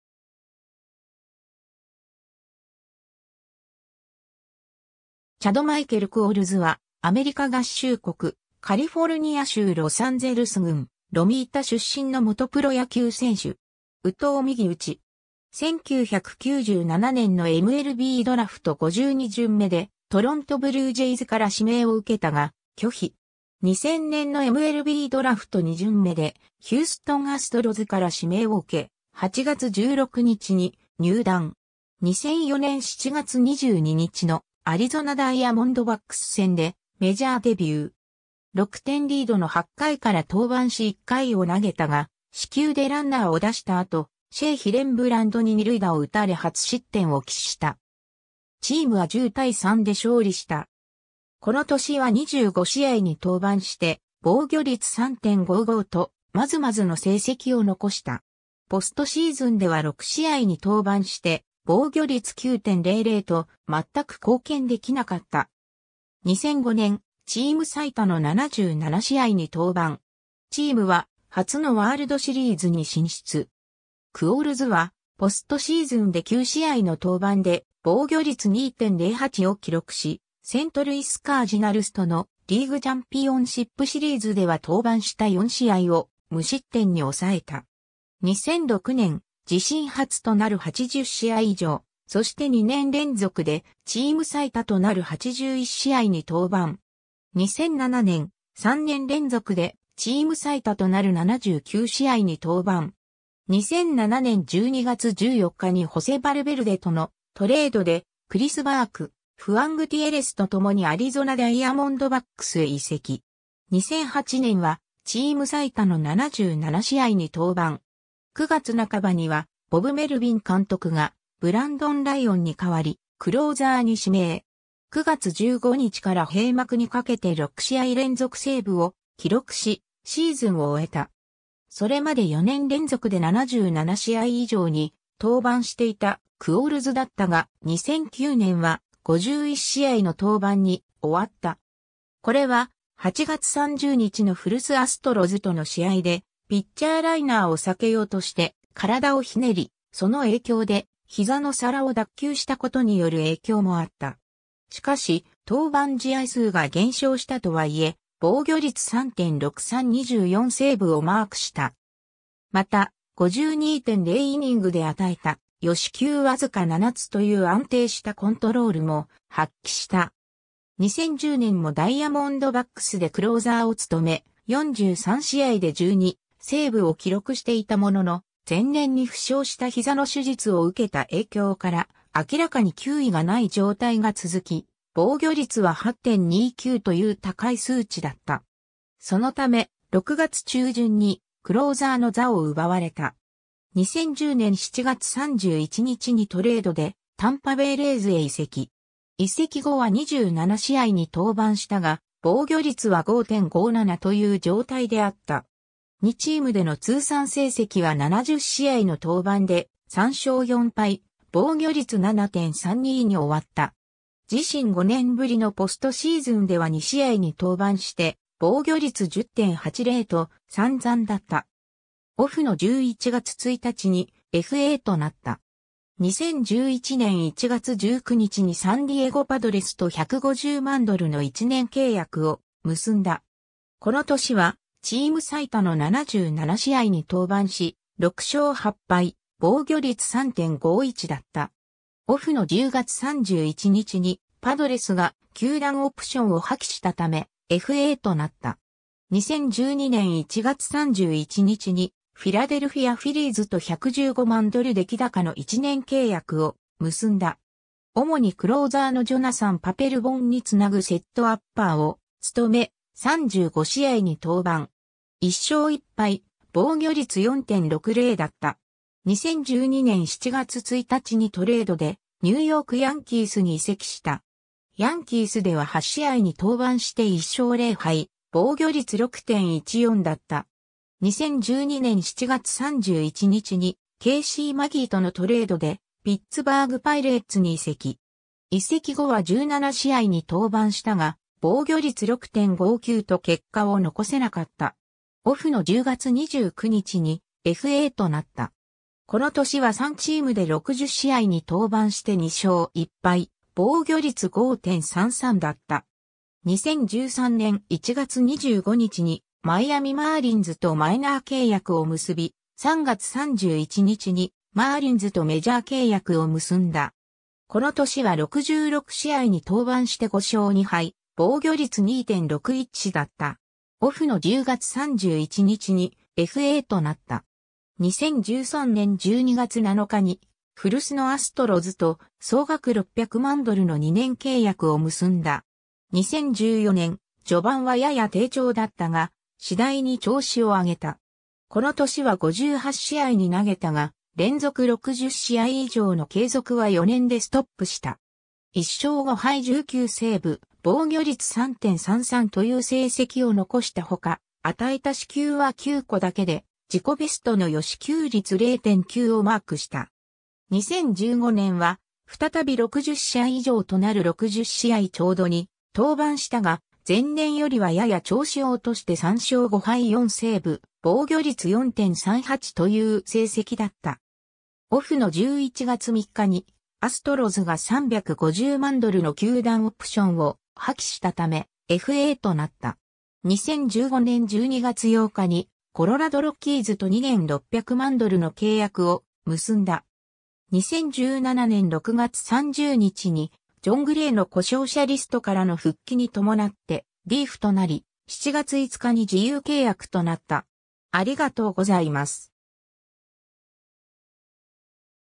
チャド・クオルズ
チャド・マイケル・クオルズ（Chad Michael Qualls, 1978年8月17日 - ）は、アメリカ合衆国・カリフォルニア州ロサンゼルス郡ロミータ出身の元プロ野球選手（投手）。
右投右打。
 1997年のMLBドラフト52巡目（全体1444位）でトロント・ブルージェイズから指名を受けたが、拒否。
 2000年のMLBドラフト2巡目（全体67位）でヒューストン・アストロズから指名を受け、8月16日に入団。
 2004年7月22日のアリゾナ・ダイヤモンドバックス戦でメジャーデビュー。
6点リードの8回から登板し1回を投げたが、四球でランナーを出した後、シェイ・ヒレンブランドに2塁打を打たれ初失点を喫した。
チームは10対3で勝利した。
この年は25試合に登板して防御率3.55と、まずまずの成績を残した。
ポストシーズンでは6試合に登板して防御率9.00と、全く貢献できなかった。
 2005年、チーム最多の77試合（リーグ8位）に登板。
チームは初のワールドシリーズに進出。
クオルズはポストシーズンで9試合の登板で防御率2.08を記録し、セントルイス・カージナルスとのリーグチャンピオンシップシリーズでは登板した4試合を無失点に抑えた。
 2006年、自身初となる80試合以上、そして2年連続でチーム最多となる81試合（リーグ6位）に登板。
 2007年、3年連続でチーム最多となる79試合（リーグ9位）に登板。
 2007年12月14日にホセ・バルベルデとのトレードで、クリス・バーク、フアン・グティエレスとともにアリゾナ・ダイヤモンドバックスへ移籍。
 2008年はチーム最多の77試合（リーグ8位）に登板。
9月半ばにはボブ・メルビン監督がブランドン・ライオンに代わりクローザーに指名。
9月15日から閉幕にかけて6試合連続セーブを記録し、シーズンを終えた。
 それまで4年連続で77試合以上に登板していたクオルズだったが、2009年は、51試合の登板に終わった。
これは、8月30日の古巣アストロズとの試合で、ピッチャーライナーを避けようとして体をひねり、その影響で膝の皿を脱臼した事による影響もあった。
しかし、登板試合数が減少したとは言え、防御率3.63・24セーブをマークした。
また、52.0イニングで与えた与四球わずか7つ（与四球率1.21）という安定したコントロールも発揮した。
 2010年もダイヤモンドバックスでクローザーを務め、43試合で12セーブを記録していたものの、前年に負傷した膝の手術を受けた影響から、明らかに球威がない状態が続き、防御率は8.29という高い数値だった。
そのため、6月中旬にクローザーの座を奪われた。
 2010年7月31日にトレードで、タンパベイ・レイズへ移籍。
移籍後は27試合に登板したが、防御率は5.57という状態であった。
2チームでの通算成績は70試合の登板で、3勝4敗・防御率7.32に終わった。
自身5年ぶりのポストシーズンでは2試合に登板して防御率10.80と散々だった。
 2011年1月19日にサンディエゴ・パドレスと150万ドルの1年契約（2012年の球団側選択オプション付き）を結んだ。
この年はチーム最多の77試合（リーグ8位）に登板し、6勝8敗・防御率3.51だった。
オフの10月31日にパドレスが球団オプションを破棄したため、FAとなった。
 2012年1月31日にフィラデルフィア・フィリーズと115万ドル+出来高の1年契約を結んだ。
主にクローザーのジョナサン・パペルボンにつなぐセットアッパーを務め、35試合に登板。
1勝1敗、防御率4.60だった。
 2012年7月1日にトレードで、ニューヨーク・ヤンキースに移籍した。
ヤンキースでは8試合に登板して1勝0敗・防御率6.14だった。
 2012年7月31日にケイシー・マギーとのトレードで、ピッツバーグ・パイレーツに移籍。
移籍後は17試合に登板したが、防御率6.59と結果を残せなかった。
この年は3チームで60試合に登板して2勝1敗・防御率5.33だった。
 2013年...